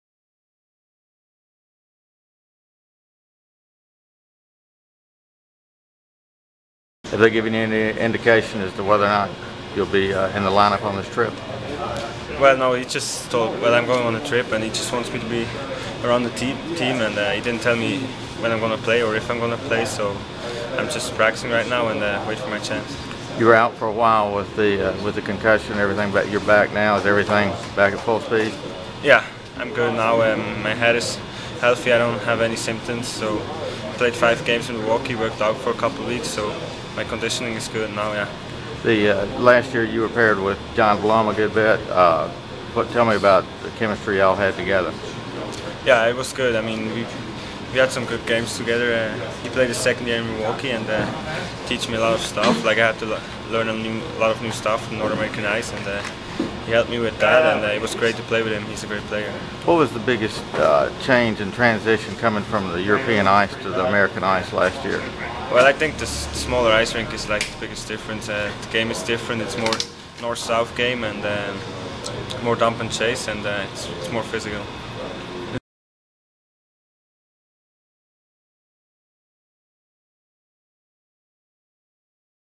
Roman Josi Speaks after his First Practice in Nashville
Predators Rookie Defenseman Roman Josi Speaks after his First Practice in Nashville on 11-25-11